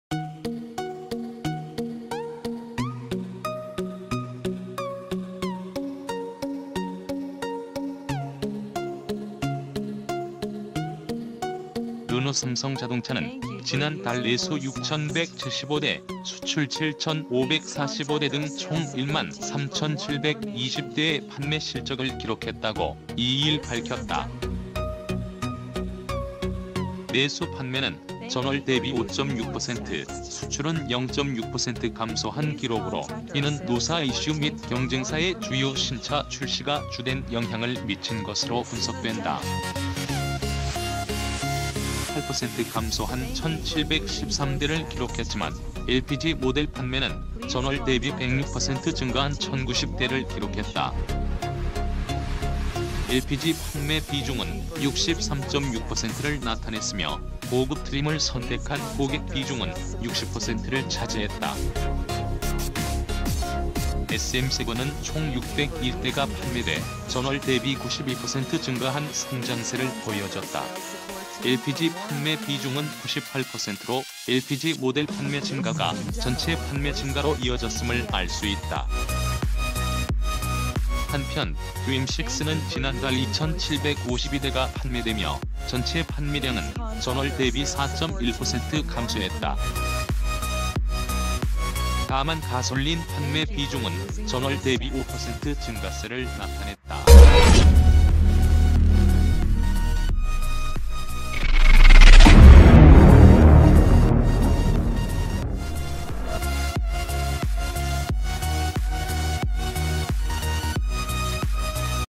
르노삼성, 4월 내수 6175대 기록..LPG 판매 ‘증가세’ |카247
,르노삼성,박홍준 기자,
르노삼성, 4월 내수 6175대 기록..LPG 판매 ‘증가세’ |카247 ► Subscribe for more !-- content_bodytext start --르노삼성자동차는 지난 달 내수 6175대, 수출7545대 등 총 1만3720대의 판매 실적을 기록했다고 2일 밝혔다.